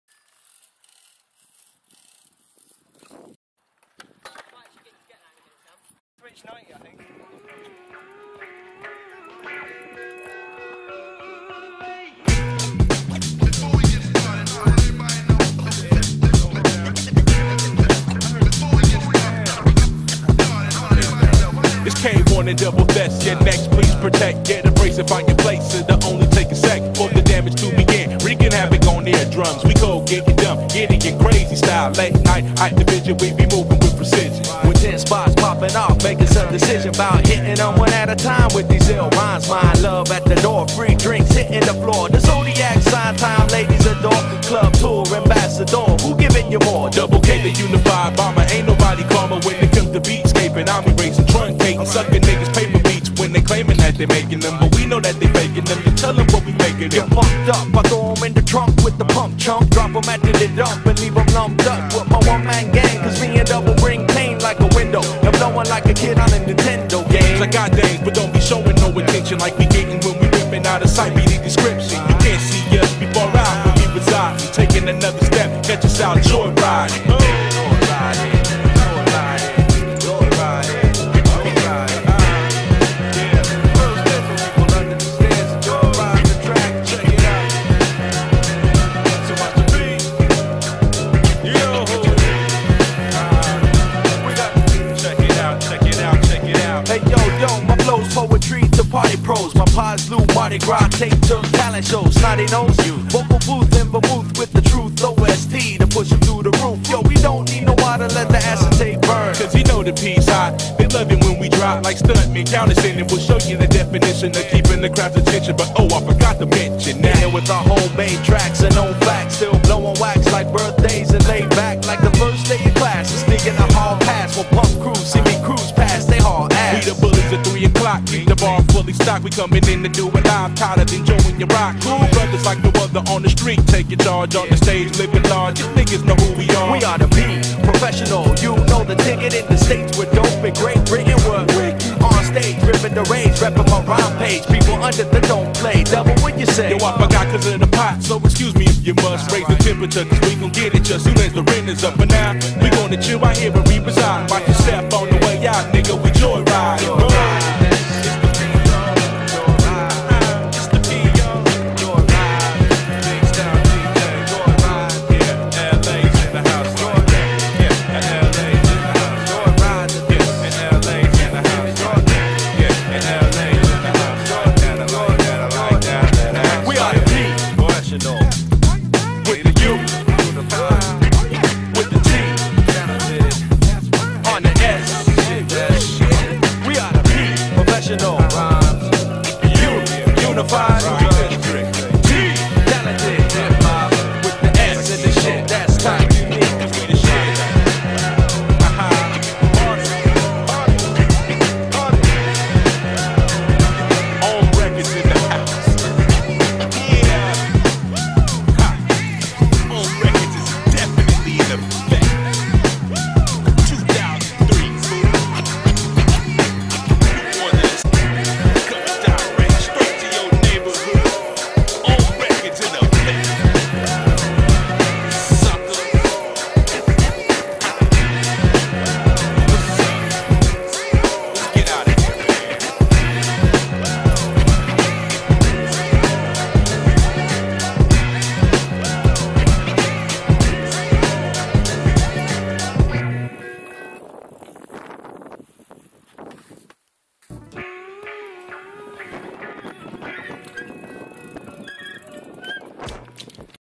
Mike Wallace - Bmx Edit
An edit of Mike Wallace and a few extras, some of this was filmed a while ago hence the shaking (: enjoy
Camera and Edit by Sam Reagan.

FROGGGGGG